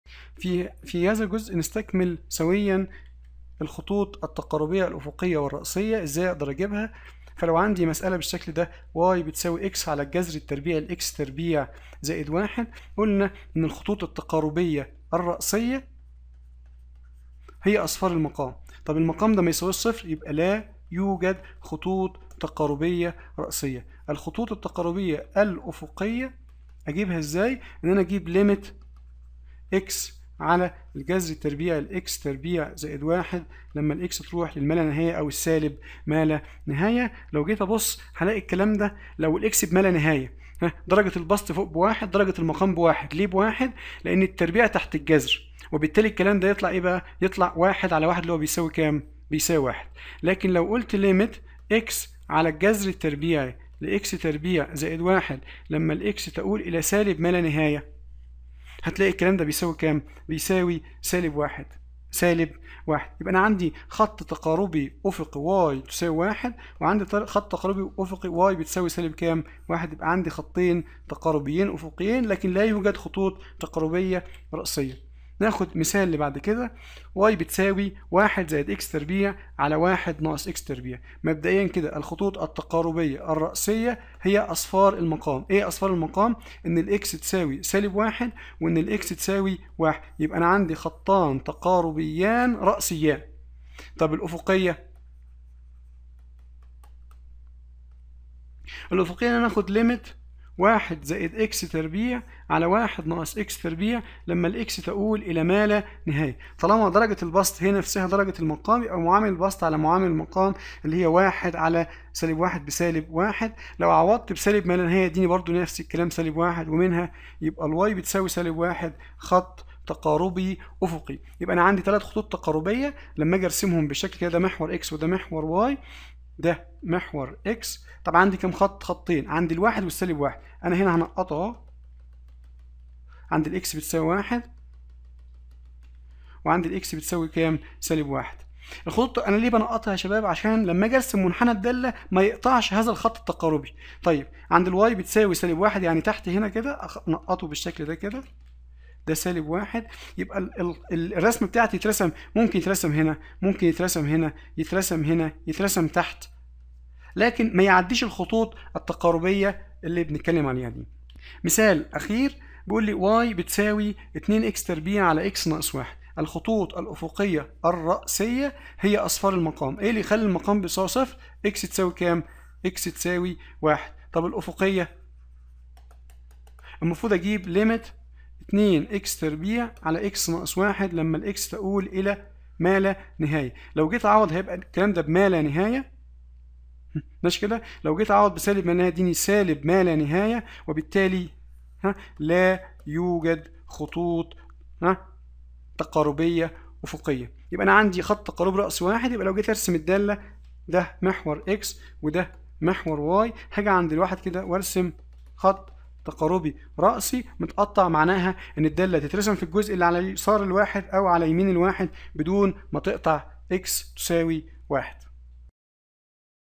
حساب التفاضل والتكامل - الوحدة 5 : الخطوط التقاربية ورسم المنحنيات - 2 - Asymptotes
المقررات المفتوحة
حساب التفاضل والتكامل(1) - 101 ريض
الوحدة الخامسة - دراسة تغير الدالة
أمثلة علي إيجاد الخطوط التقاربية الأفقية - الخطوط التقاربية الرأسية.